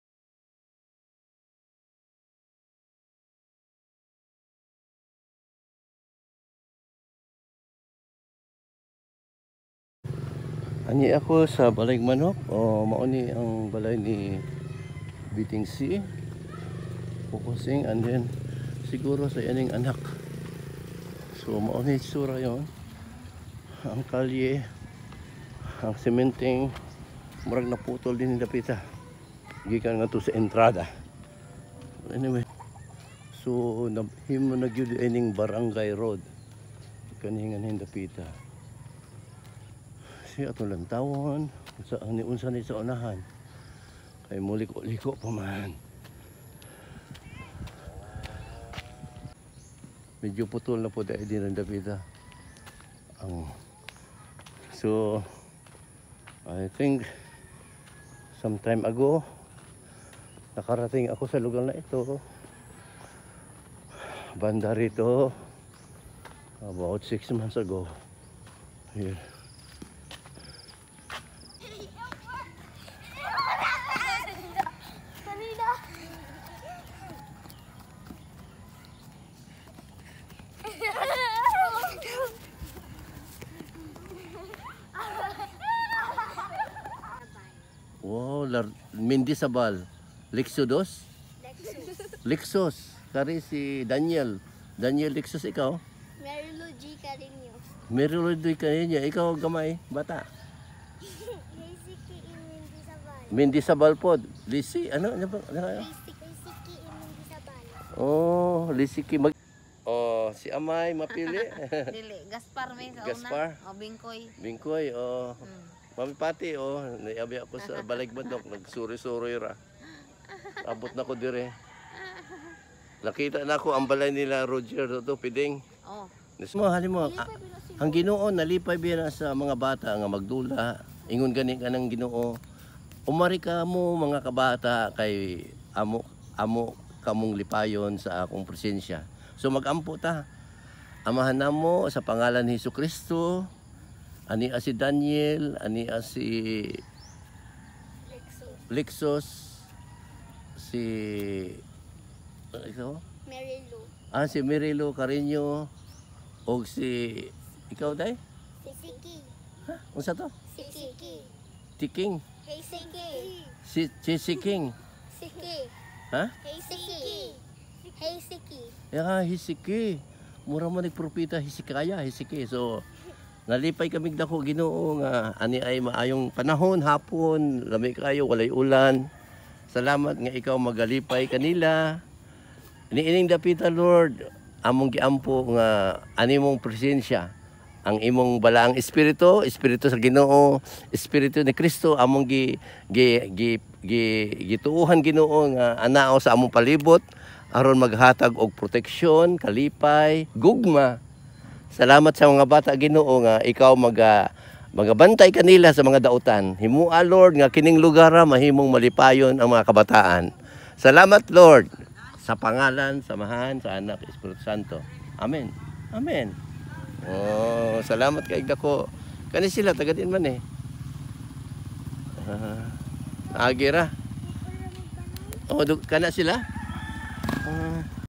27Feb’24 Balayagmanok Children Encounter with a Neighbor Aunt
Praying for them after they play